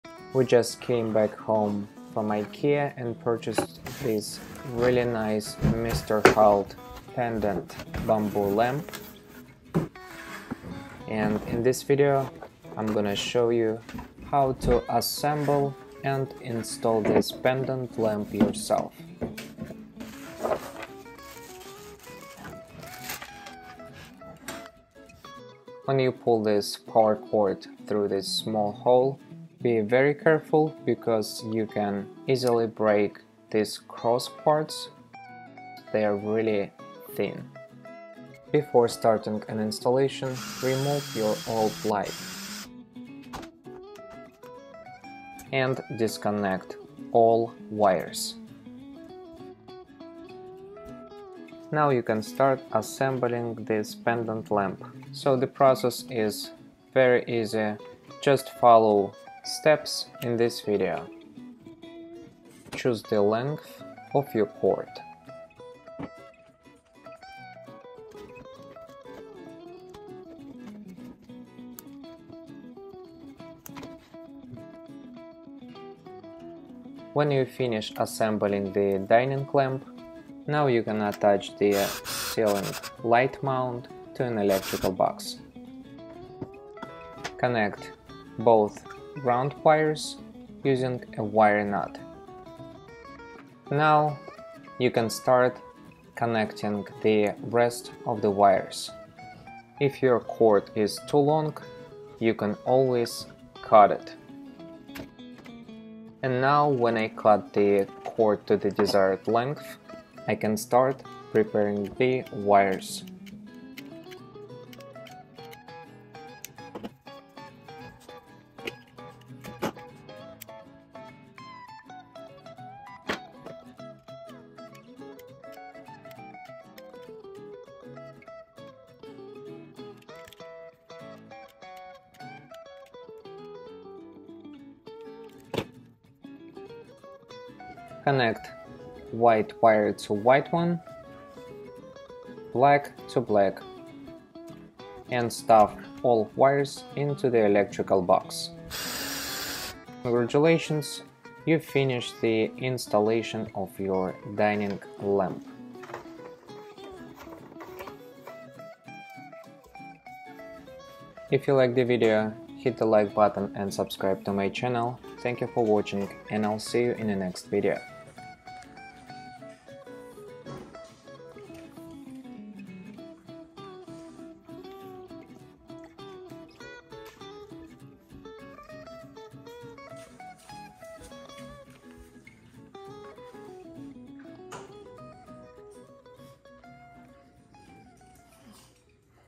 How to Assemble and Install IKEA Misterhult Bamboo Pendant Lamp
Hi,
In this video I will show you How to Assemble and Install IKEA Misterhult Bamboo Pendant Lamp.